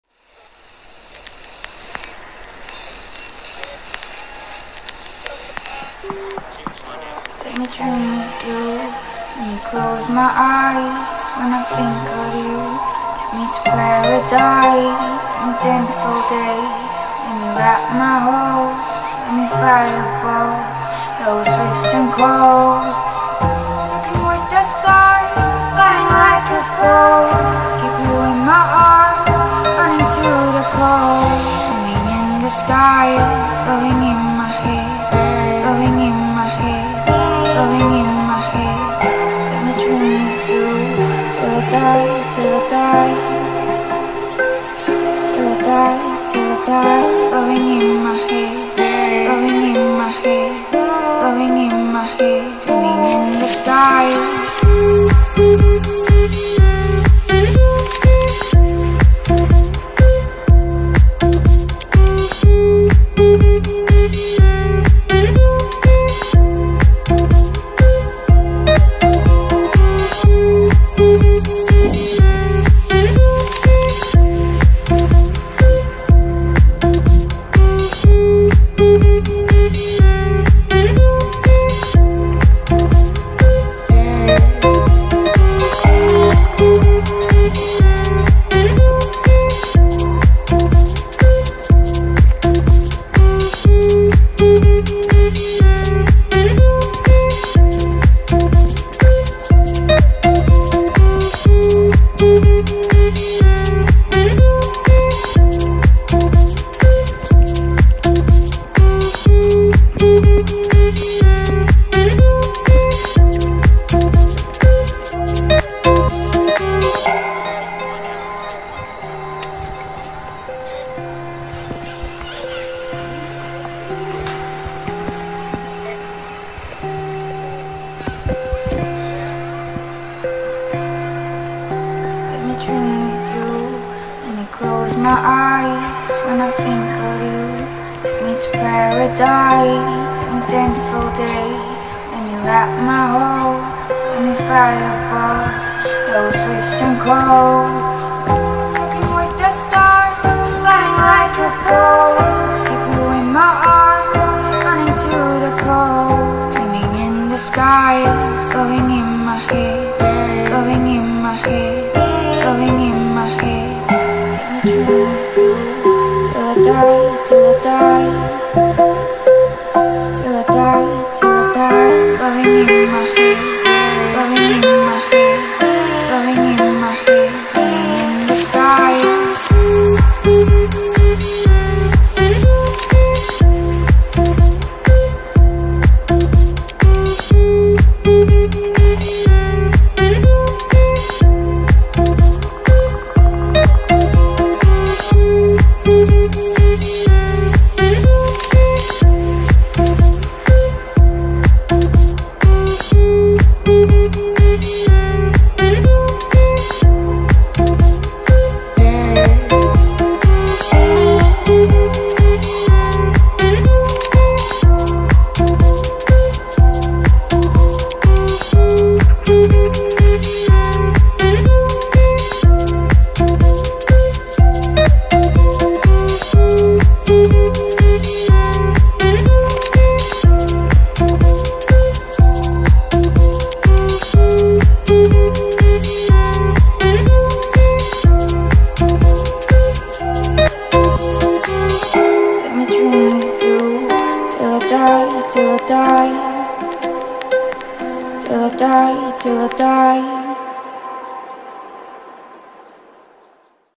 Russian River
Such a beautiful day. Black berries picking while Paddle boarding and Kayaking at Russian River.